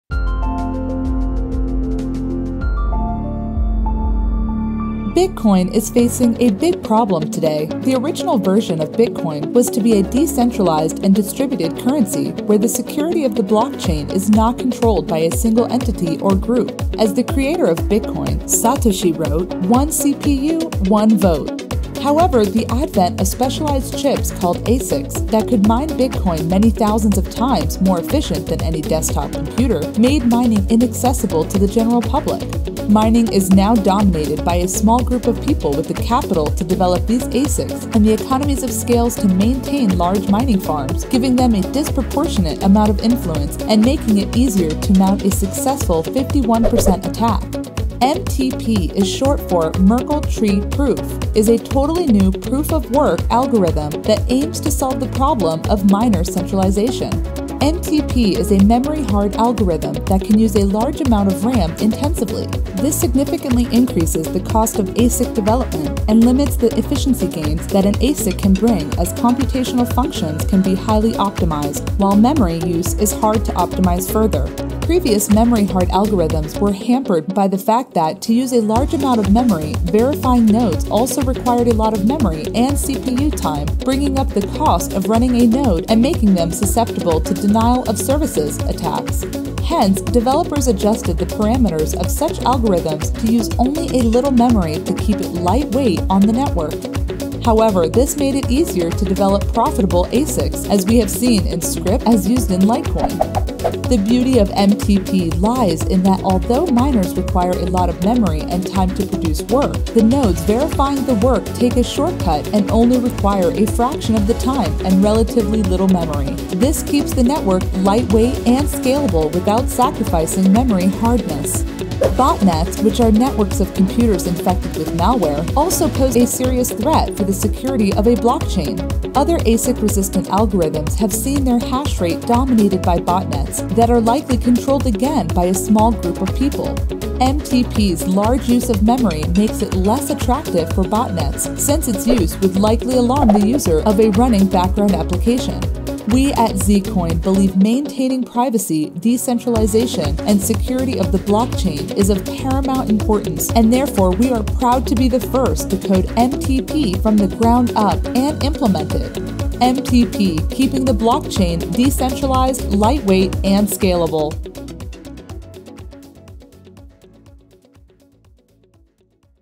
Merkle Tree Proof (MTP) Algorithm in Zcoin
Zcoin is the first cryptocurrency to successfully code MTP from the ground up and implement it. MTP is an algorithm that offers a memory hard proof of work that remains lightweight to verify thus promoting a decentralized and scalable network.